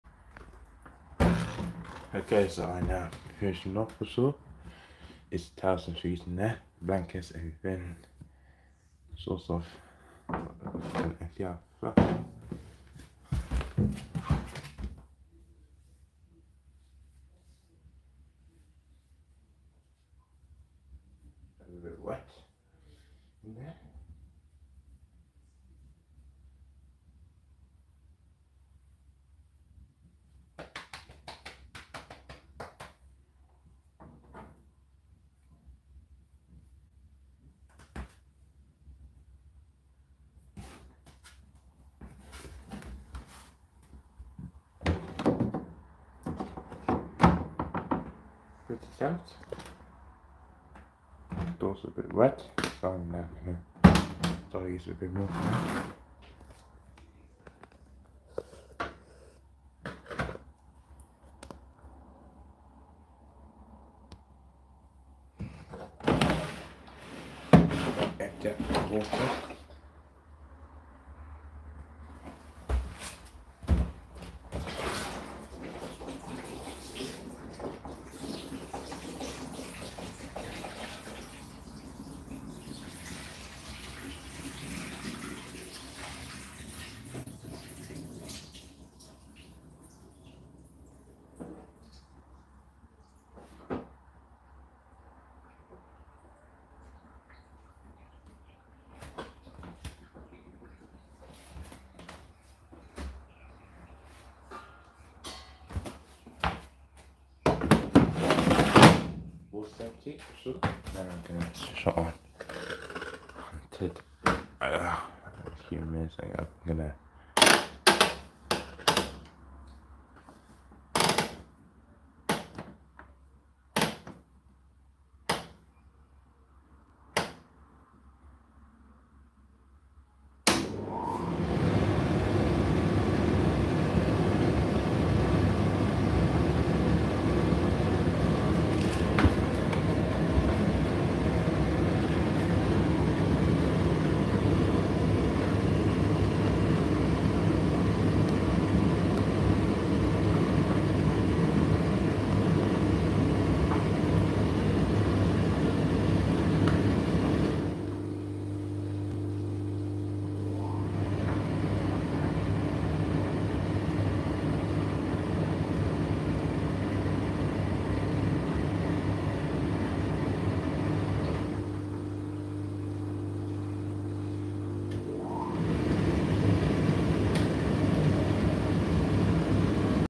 Highlight 0:00 – 3:00 from Indesit IDC8T3 8KG Condenser Tumble Dryer Full Cycle
I'm finishing off our blanket sheets.